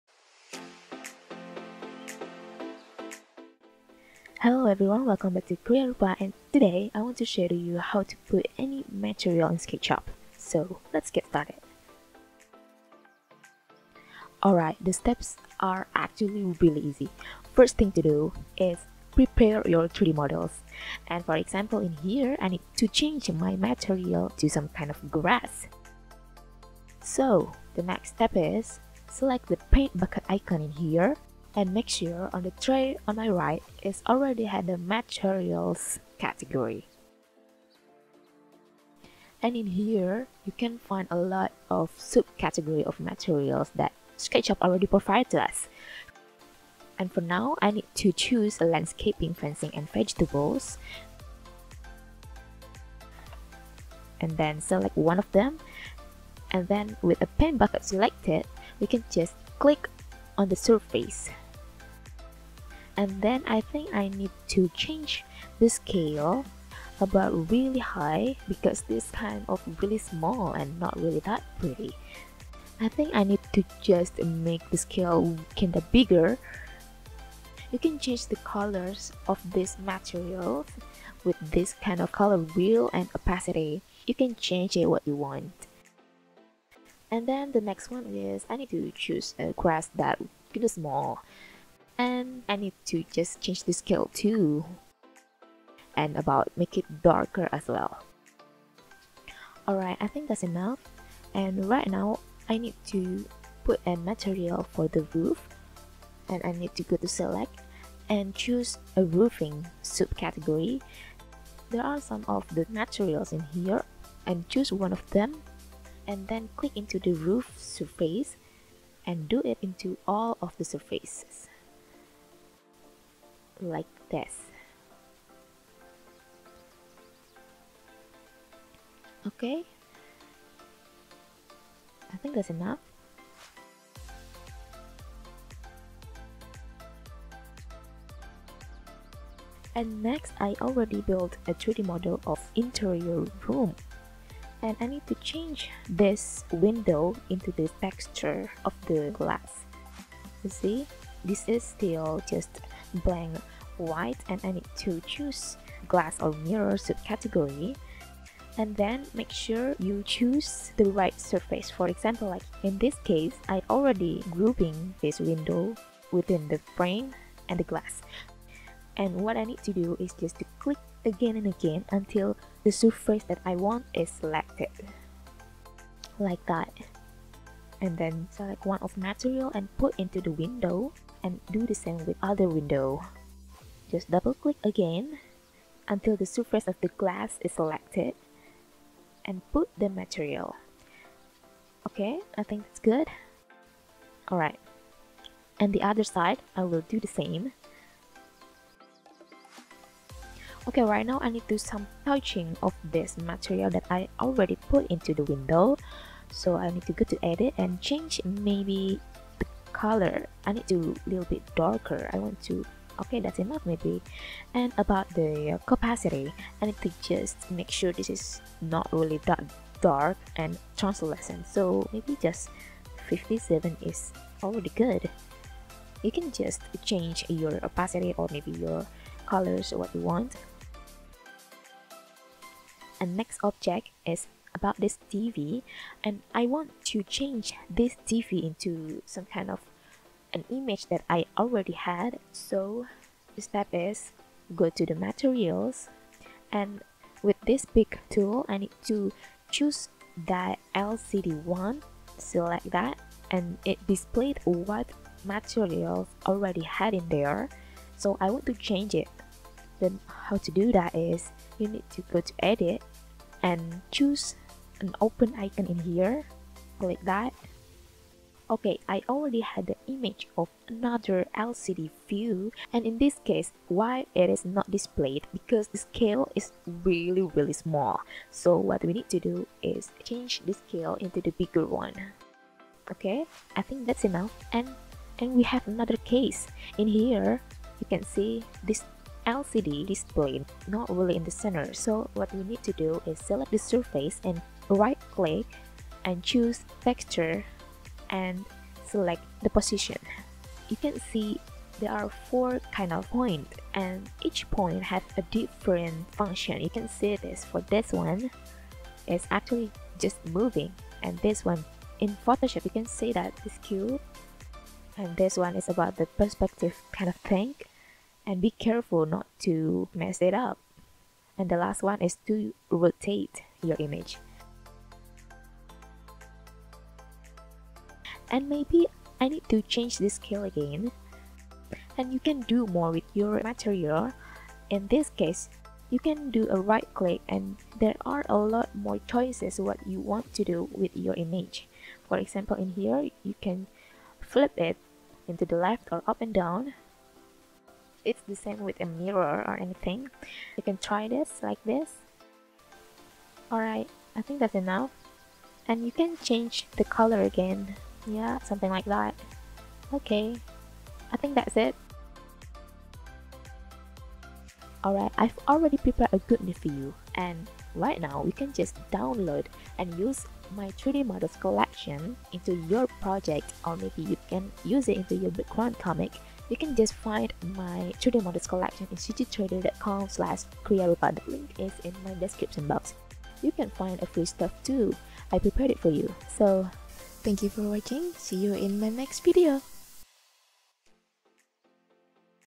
How to PUT MATERIALS in Sketchup 3D Model
[EASY Tutorial]
How to Put Materials or Texture in your 3D Model [Sketchup 3D] EASY Tutorial

Hello! Thank you for asking me in the comment section on my previous videos. Some of you asked how to put and sync the materials in Sketchup, well in this video i'll show you some EASY ways to do that. (Beginners are welcomed!)

Programs i used:
- Sketchup